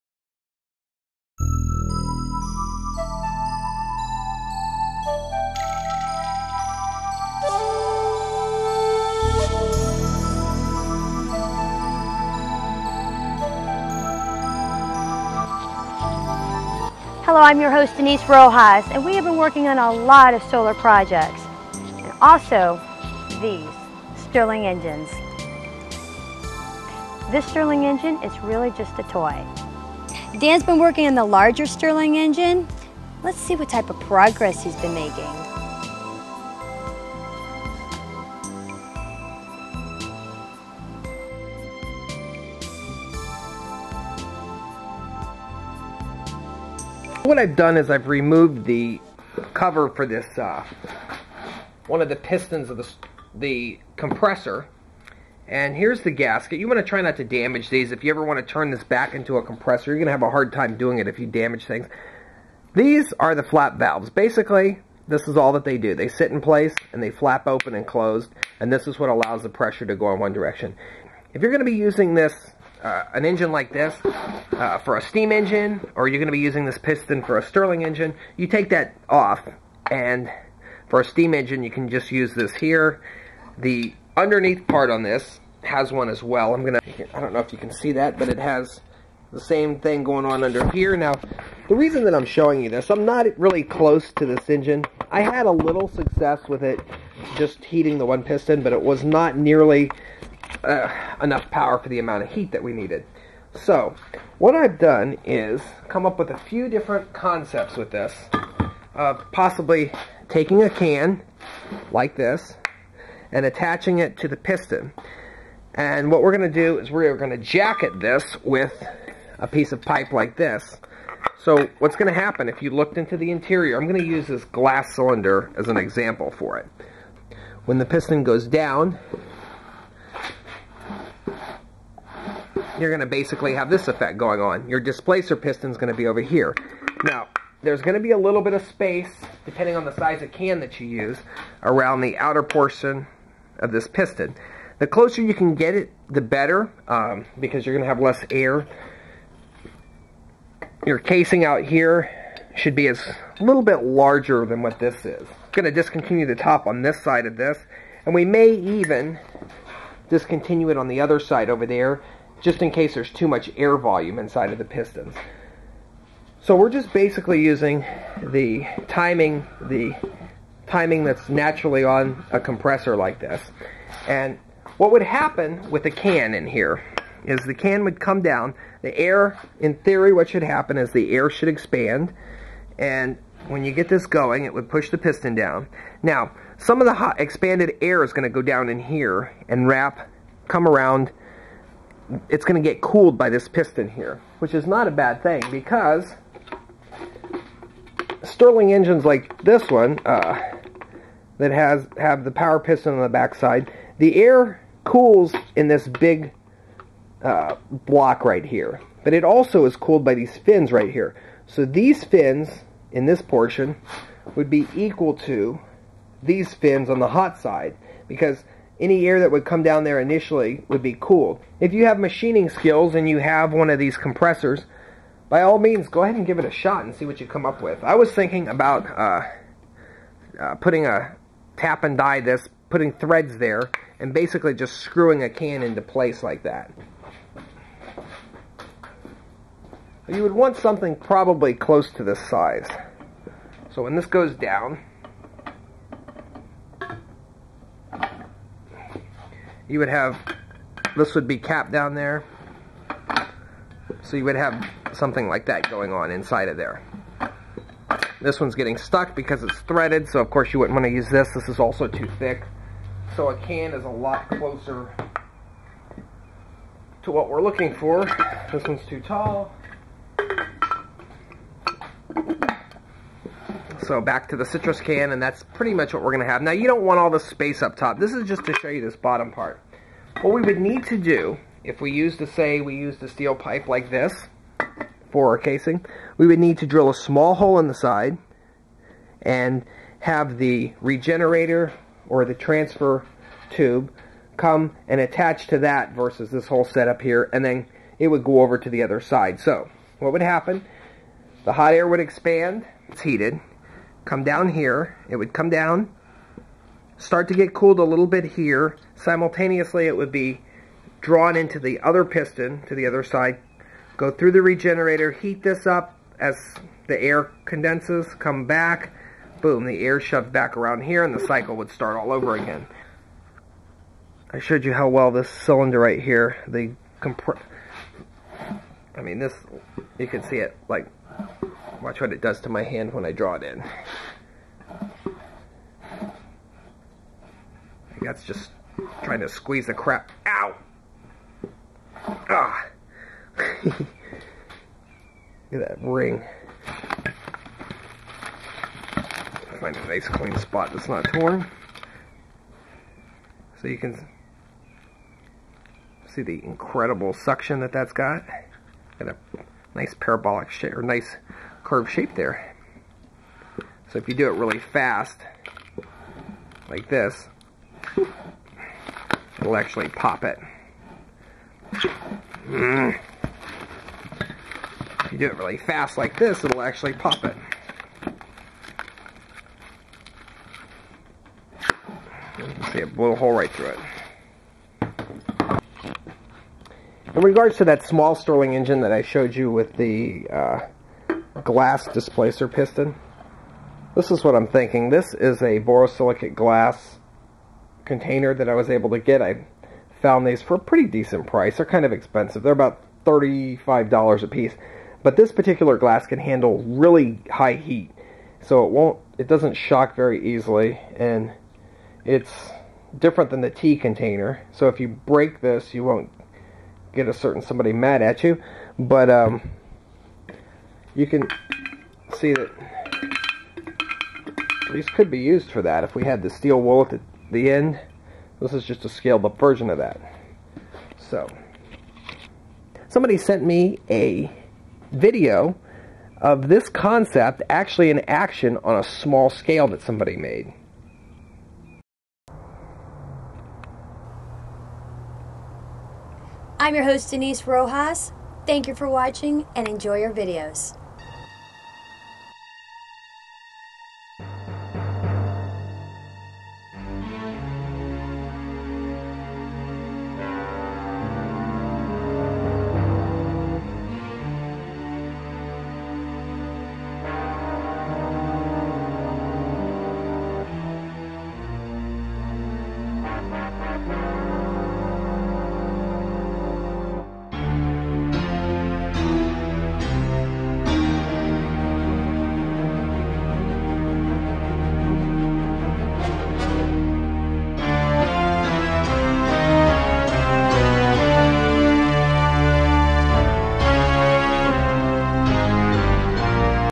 Stirling Engine Solar Compressor Conversion for Solar Training
SOLAR POWER TRAINING COURSE This is a follow-up STIRLING CYCLE ENGINE video to the Compressor Conversion. The original design required too much heat on the displacer side due to excess volume inside the piston. By adding a large displacer, you reduce the working gas volume and dramatically increase the surface area to the heat source. The piston attached to the displacer is exposed to 80% less heat than the previous design preventing damage to the rings, also reducing wear and lubricant breakdown. The heat sinked cylinder on the hot side is also saved from heat damage.

The